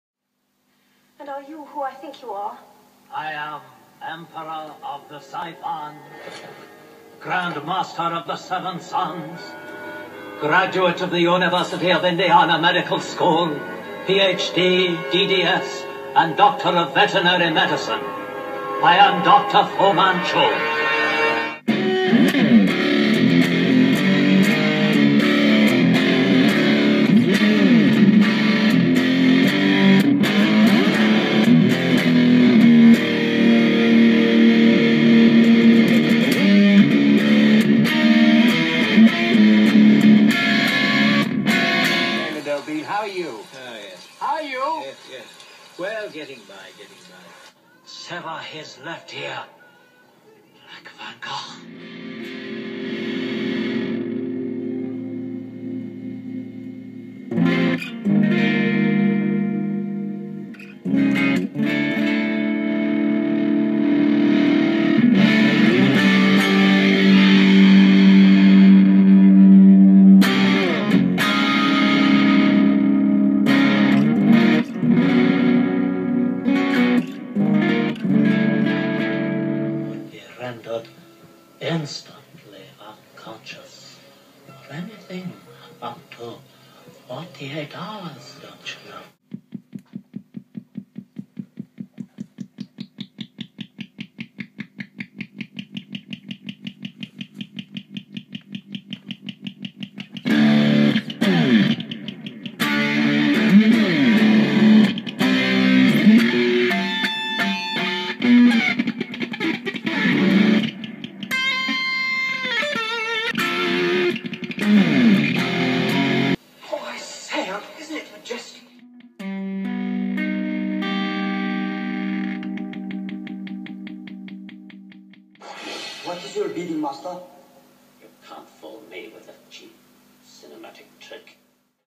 Fluxmachines - Dr. FU 2 - Elixir Vitae - oscillation fuzz w/sweet saturation
guitar effect pedals - made in germanium©
DISTORSIONE CREMOSA - 100%organic

" i need some art on my pedalboard ."